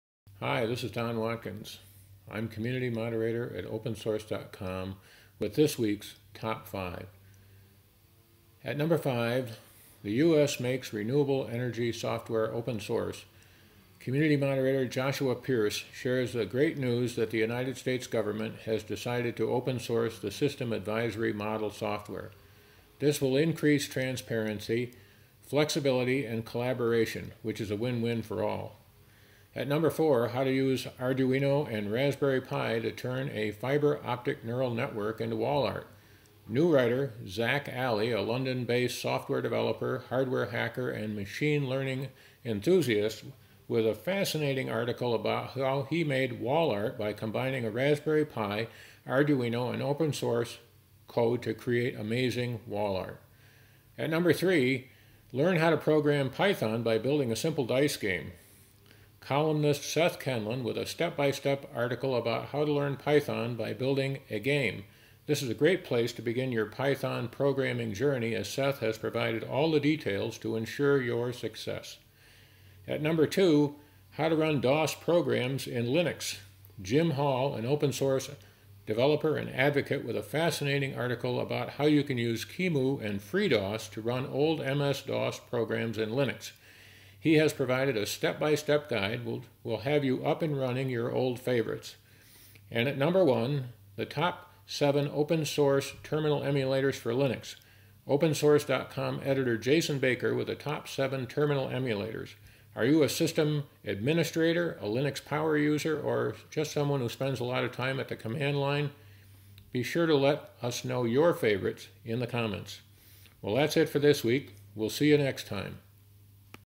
Top 5: Arduino and Raspberry Pi-powered wall art, Linux terminal emulators, and more
5. U.S. makes renewable energy software open source

4. How to use an Arduino and Raspberry Pi to turn a fiber-optic neural network into wall art

3. Learn how to program in Python by building a simple dice game

2. How to run DOS programs in Linux

1. Top 7 open source terminal emulators for Linux